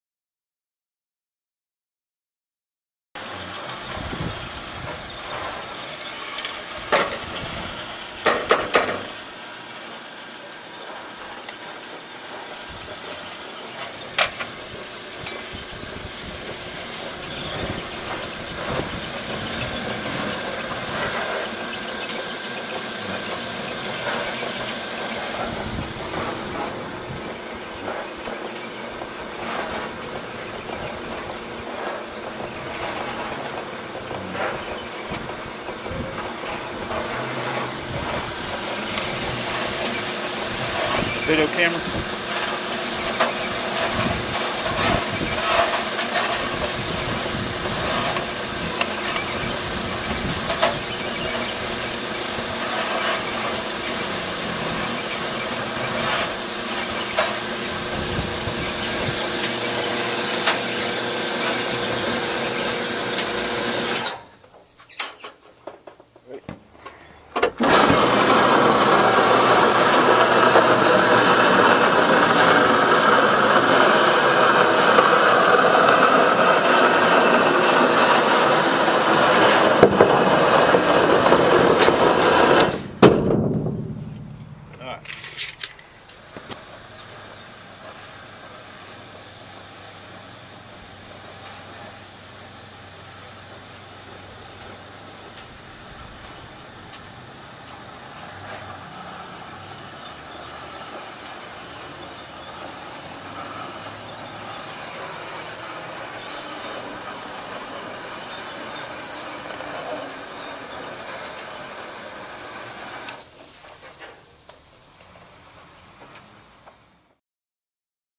Gutter Machine making new continous gutters for our home
House has gutters for the first time in years.